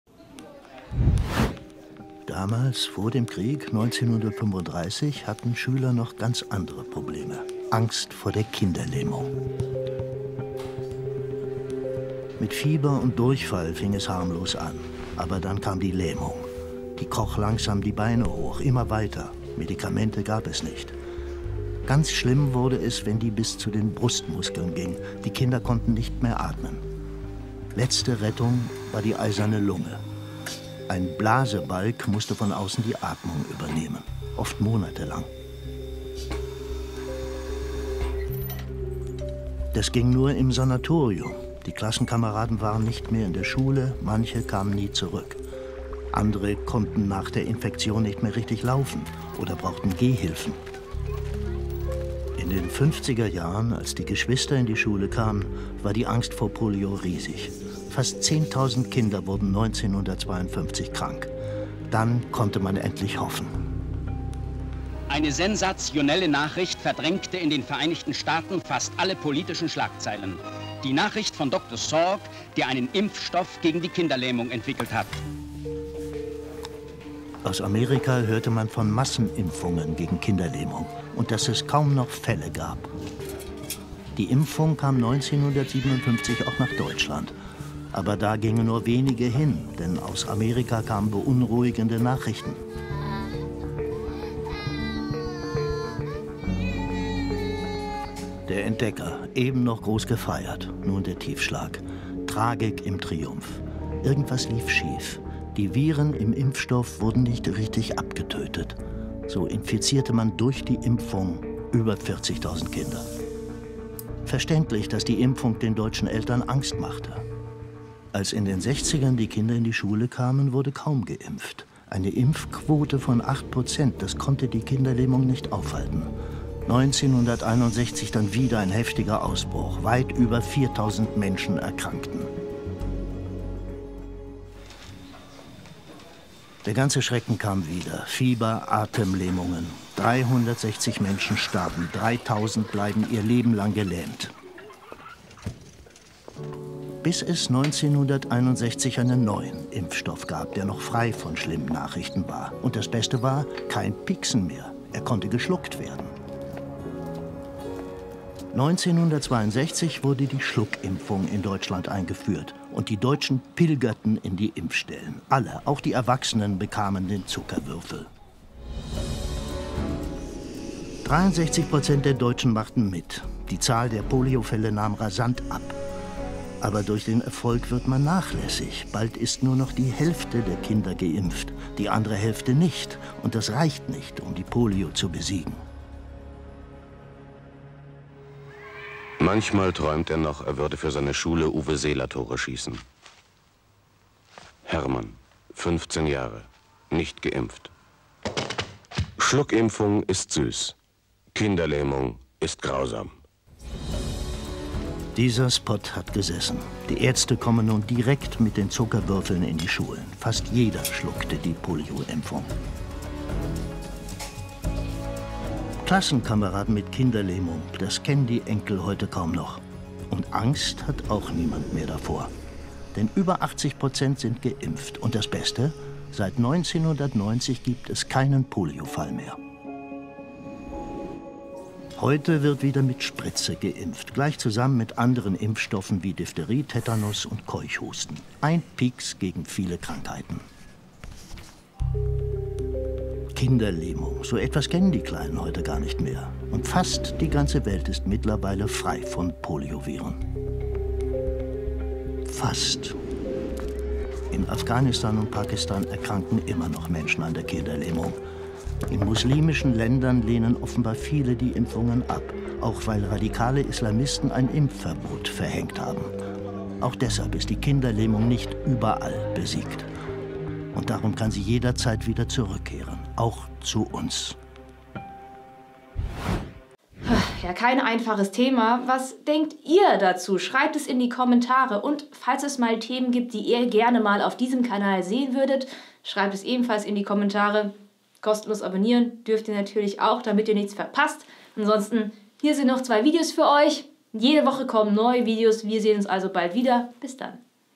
Polio: So schützt die süße Impfung Kinder auf der ganzen Welt | Quarks
Seit 1990 gab es in Deutschland keinen einzigen Polio-Fall mehr. Das ist durch rechtzeitiges Impfen zu verdanken. Trotzdem ist die Gefahr nicht vollständig gebannt, denn in anderen Ländern erkranken Menschen auch heute noch an Kinderlähmung.

Autorin: Katrin Krieft